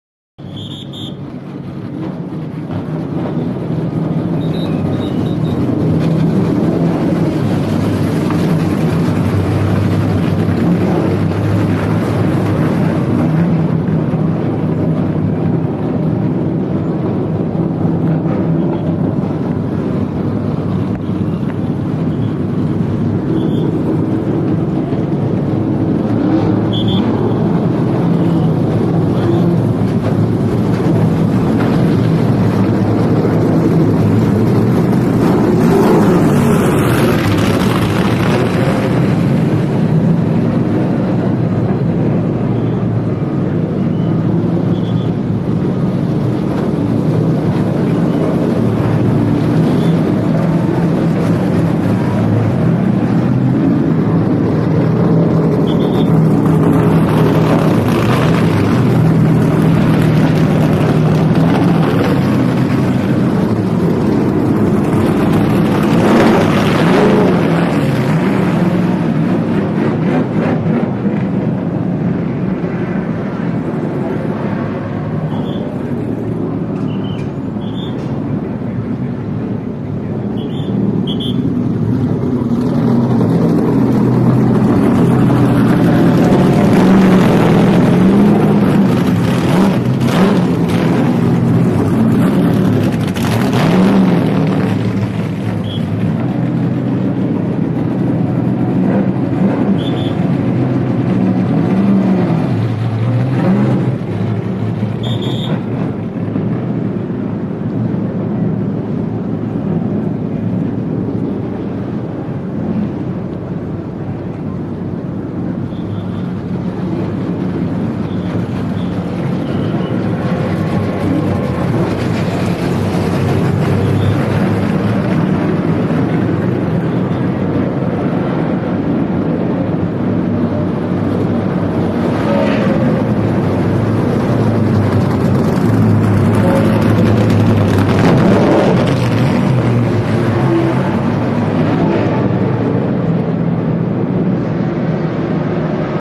360 Look at the Sprint Cup garage at Phoenix International Raceway
Take a 306 look at the Sprint Cup garage at Phoenix International Raceway.